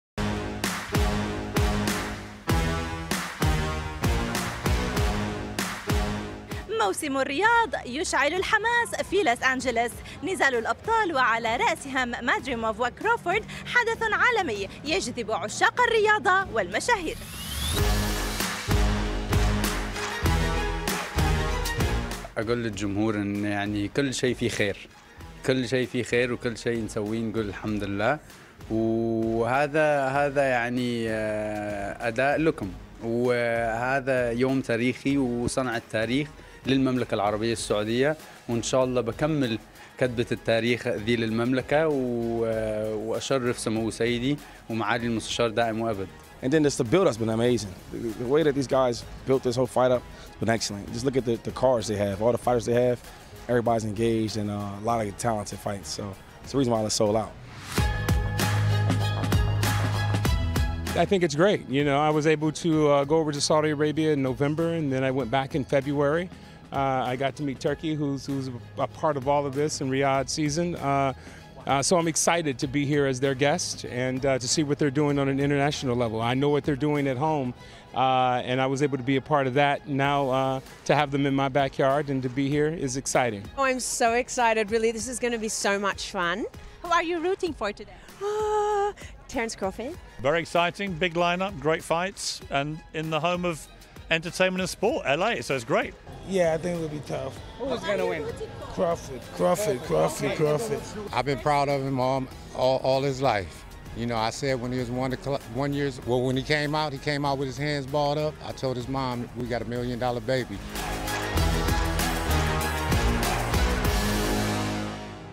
حدث عالمي يجذب عشاق الرياضة في لوس أنجلوس برعاية موسم الرياض
حدث عالمي يجذب عشاق الرياضة في لوس أنجلوس برعاية

لمعرفة أحدث المستجدات.. عبر البث المباشر من هنا 👇
وبإمكانكم متابعة العربية على منصات التواصل